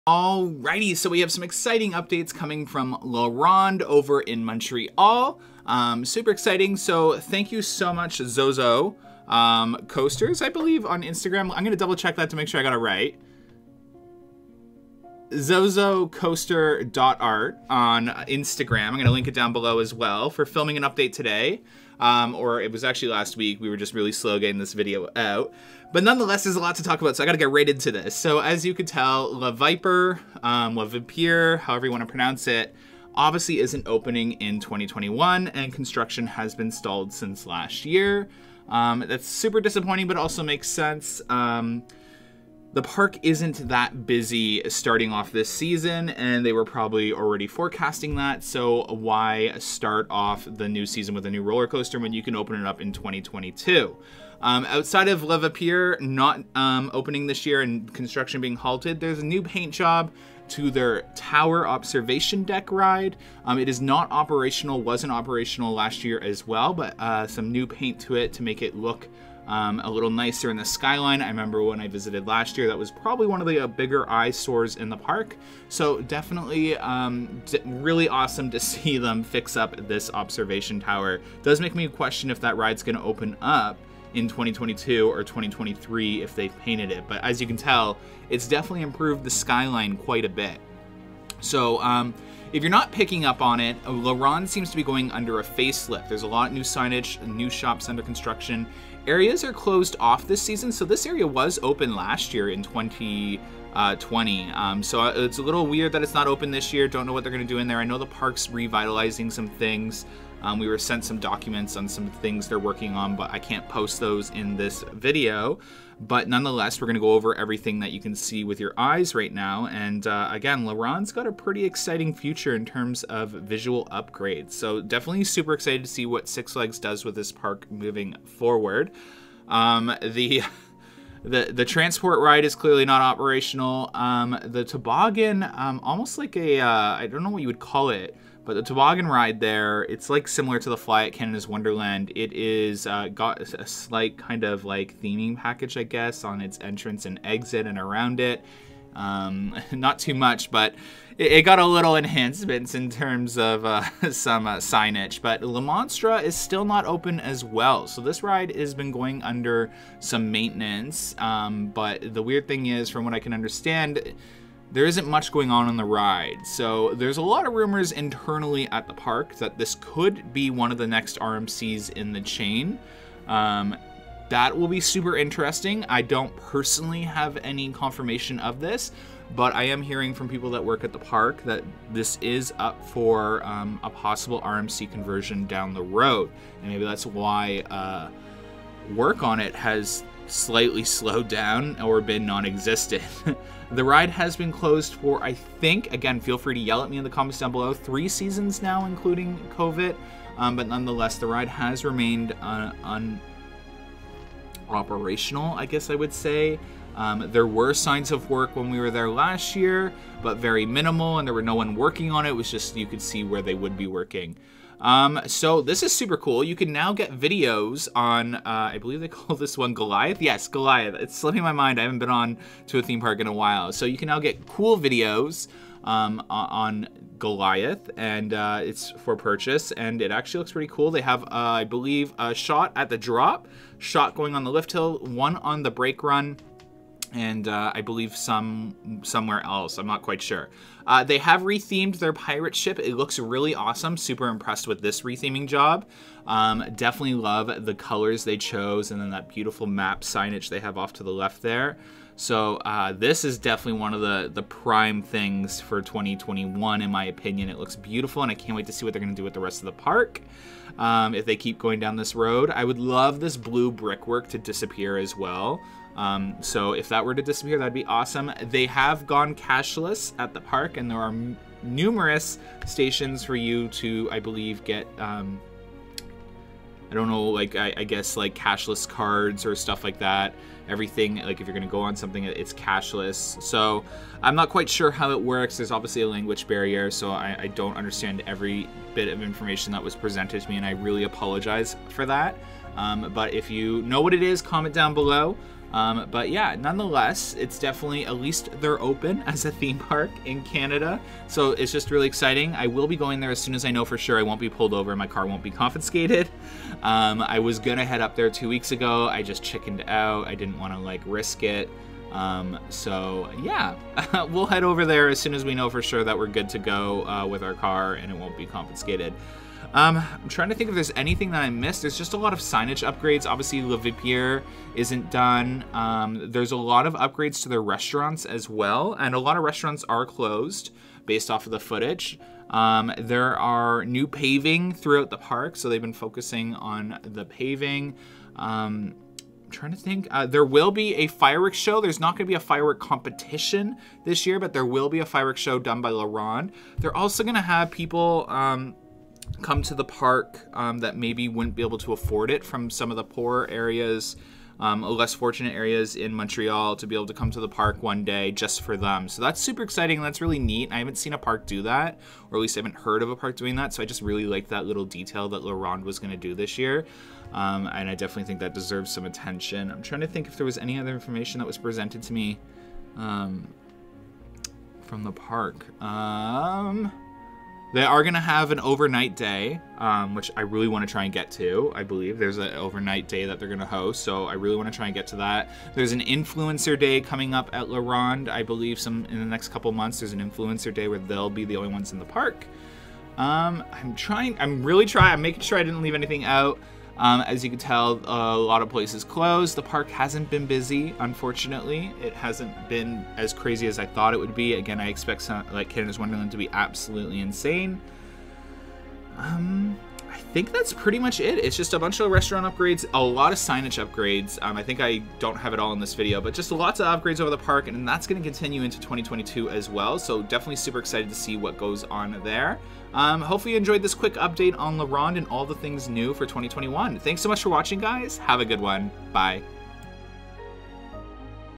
Everything New Six Flags La Ronde
Everything new at Six Flags La Ronde in 2021.
Filmed By: Zozocoaster.art on Instagram.